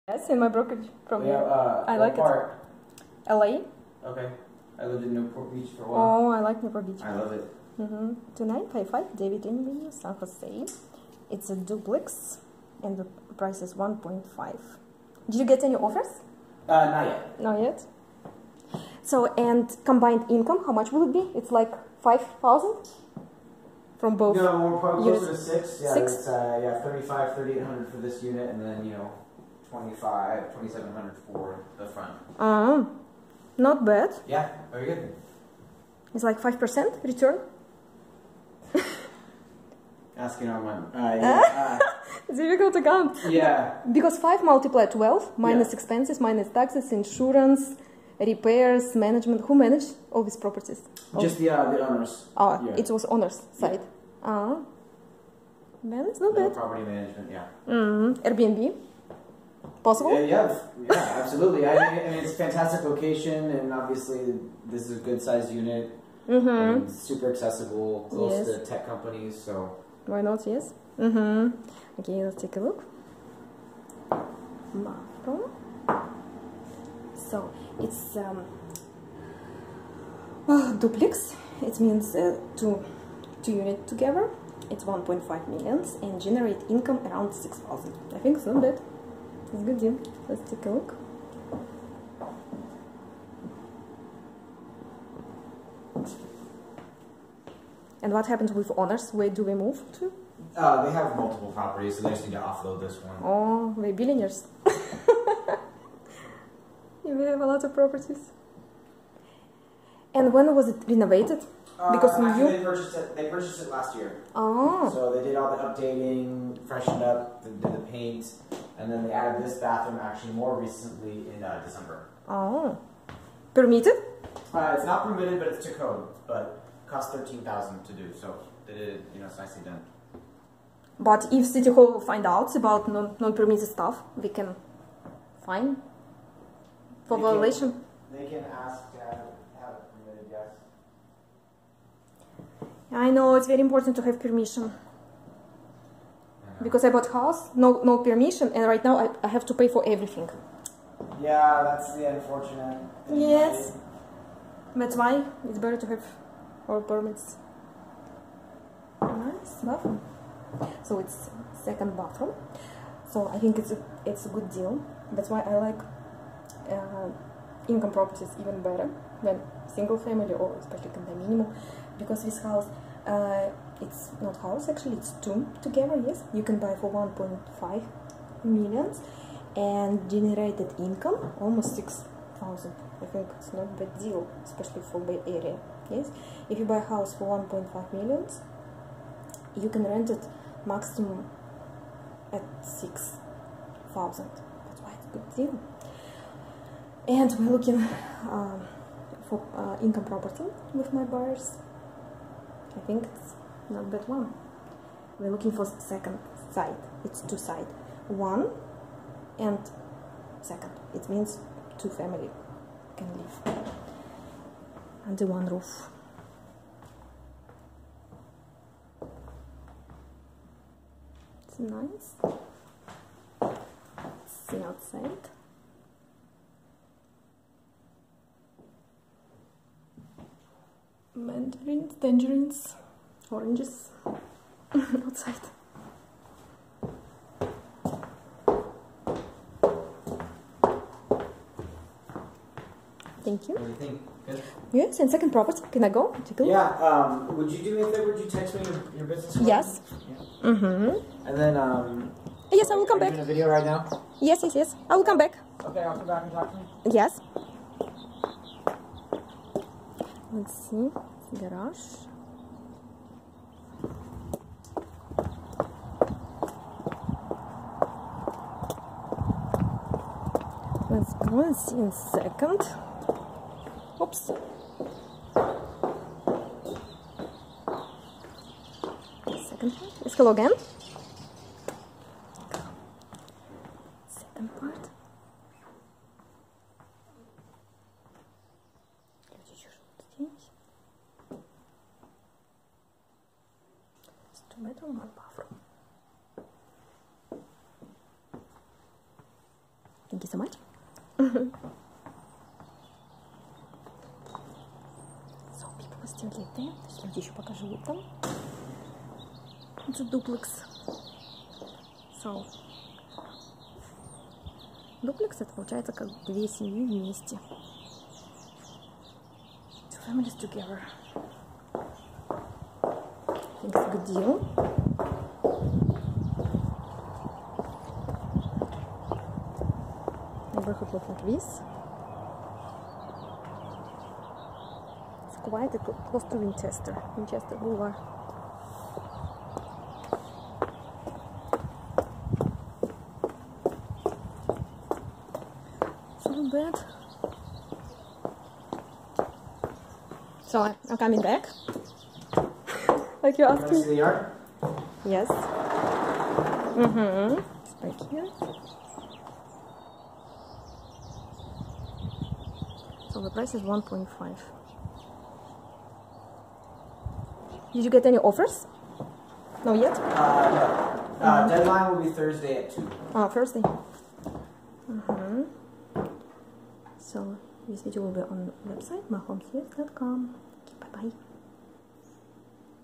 2955 David Ave, San Jose, CA 95128 $1,498,0005 bd3 ba2,182 sqft
Beautiful Multi-family home in prime location with Campbell Schools! Live in one unit and rent out the other, great for young family or young working professionals! Each home has separate entrance, separate gas and electric meter, 1 car garage, and own private yard! Centrally located to shopping plazas, Downtown Campbell, Pruneyard, eBay, Netflix, Apple Park and proposed Google Town!